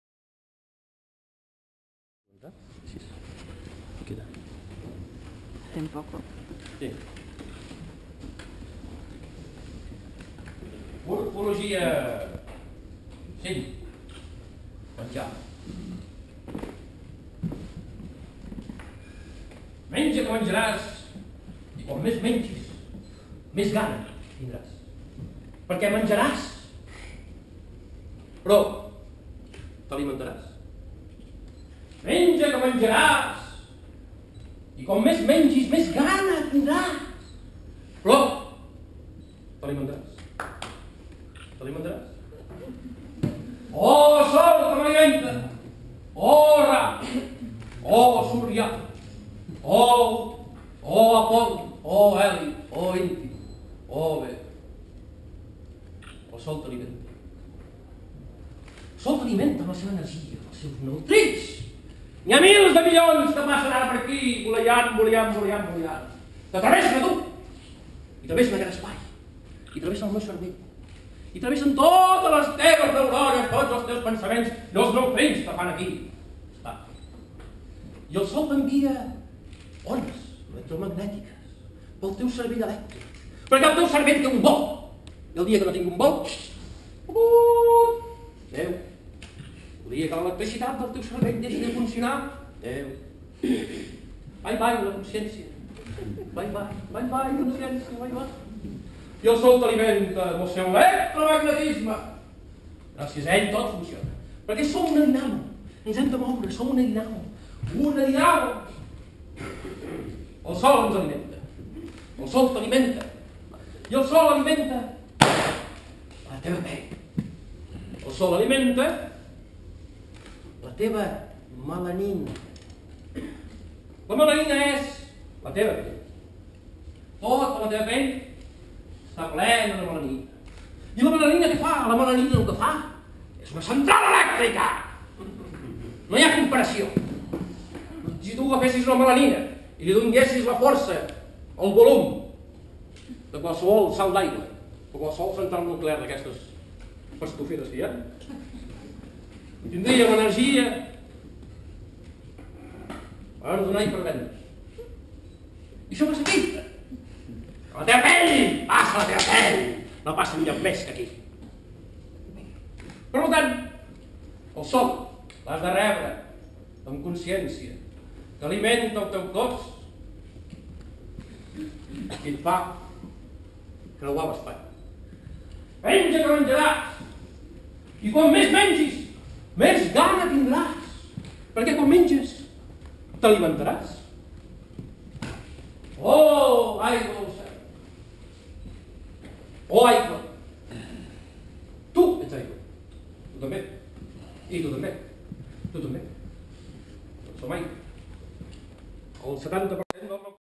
5 CORPOLOGIAw
Fragment de l'actuació celebrada el 28 de setembre del 2012 al Antic Teatre de Barcelona.
Cos i Acció és el motiu de Corpología una trobada d'artistes en acció.